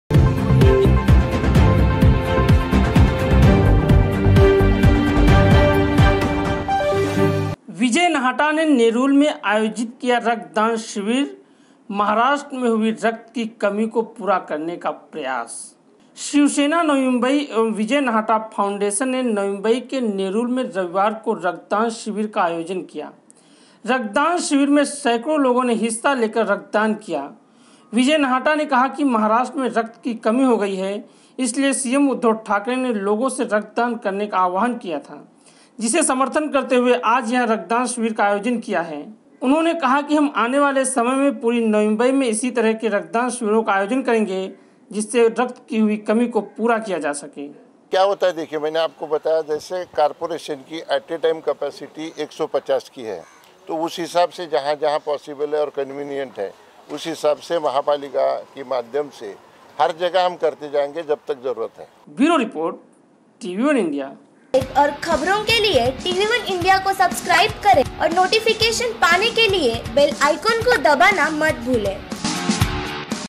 VIJAY NAHATA ने नेरूल में आयोजित किया रक्तदान शिविर
शिवसेना नवी मुंबई एवं विजय नहाटा फाउंडेशन  ने नवी मुंबई के नेरूल में रविवार को रक्तदान शिविर का आयोजन किया ... रक्तदान शिविर में सैकड़ों लोगों ने हिस्सा लेकर रक्तदान किया ... विजय नहाटा ने कहा कि महाराष्ट्र में  रक्त की कमी हो गई है इसलिए सीएम उद्धव ठाकरे ने लोगों से  रक्तदान करने का आव्हान किया था